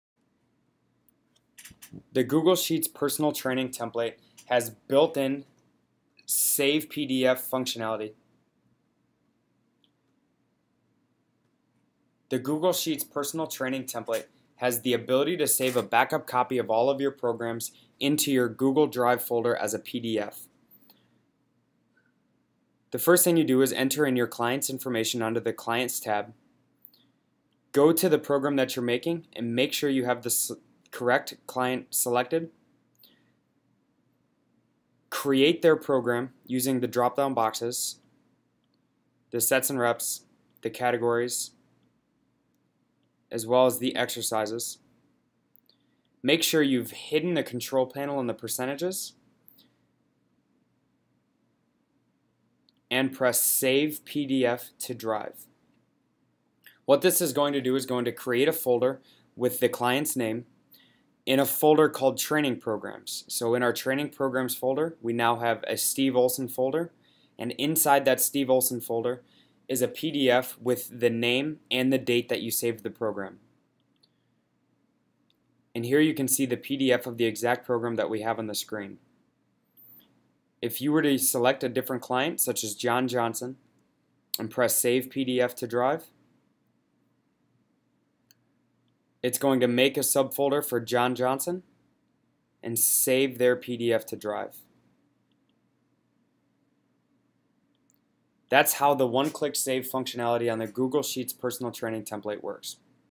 1 Click Save to PDF WIth Google Sheets Personal Training Templates
With this google sheets personal training template, you can save a backup PDF copy of the workout directly to your google drive account, into a customized folder for that client, with 1 click of a button!